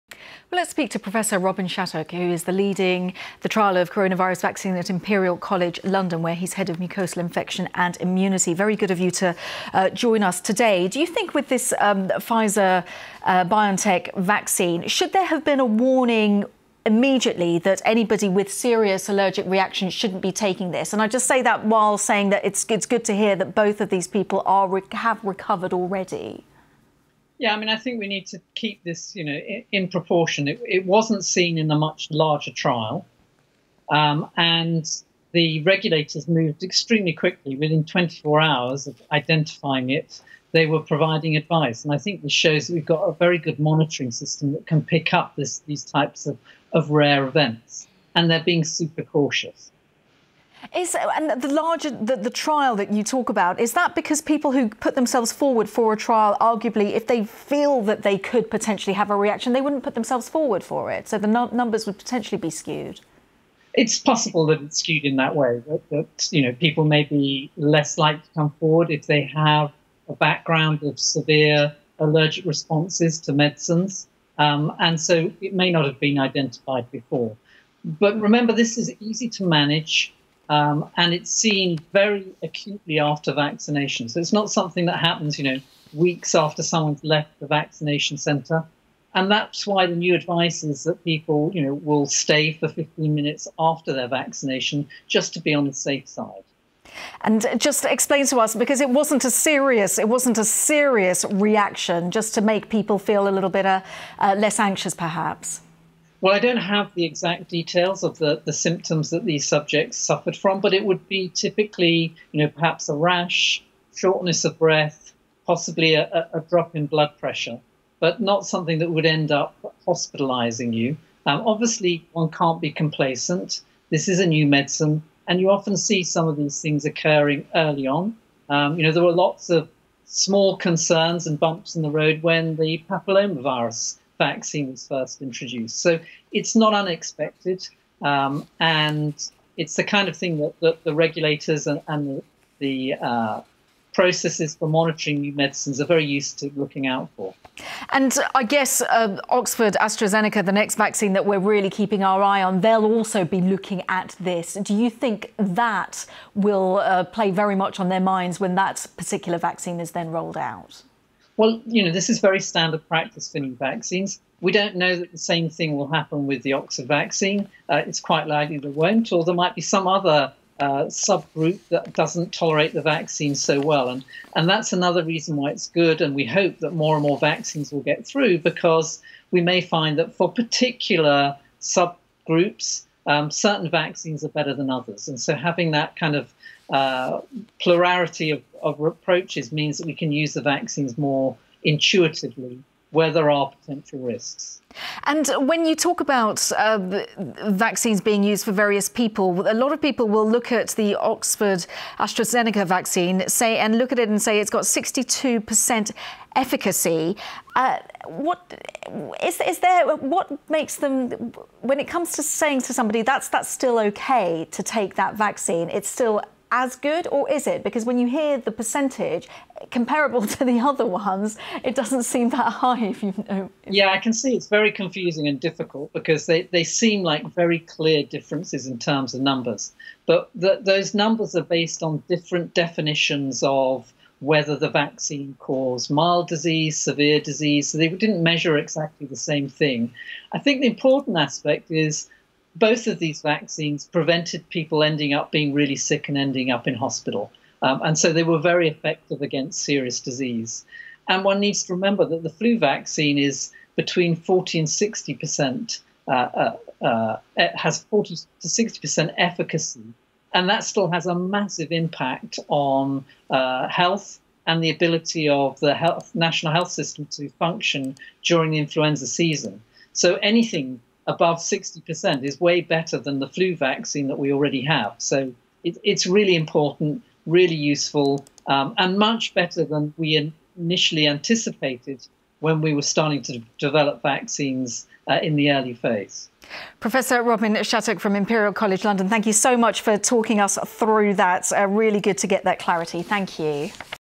Professor Robin Shattock responds to the allergy warning of the new coronavirus vaccine
Professor Robin Shattock, Head of Mucosal Infection and Immunity, who is leading the trial of a coronavirus vaccine at Imperial College London, responds to the allergy warning of the new coronavirus vaccine